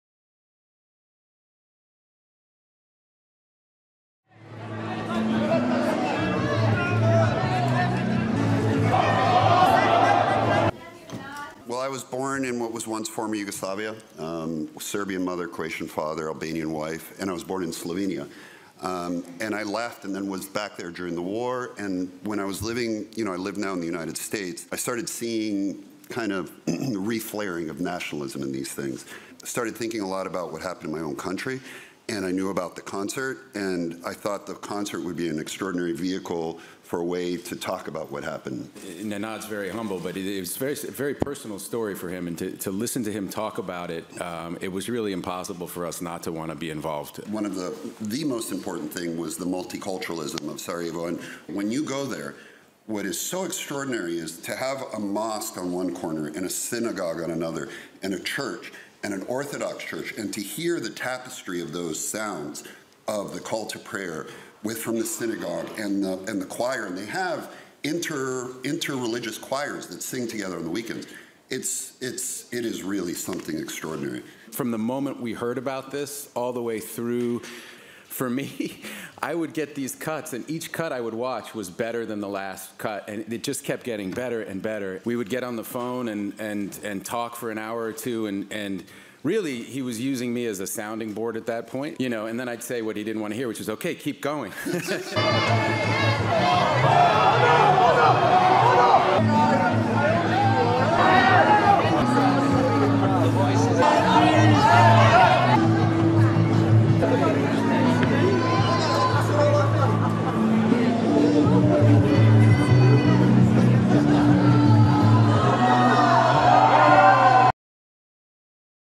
KISS THE FUTURE Berlinale 2023 Nenad Cicin Sain Matt Damon Bono
Take a director born to a Serbian mother, and a Croatian father in Slovenia, with an Albanian wife, add Matt Damon and Bono to the mix, place it in Sarajevo and you get "Kiss The Future", a documentary about nationalism.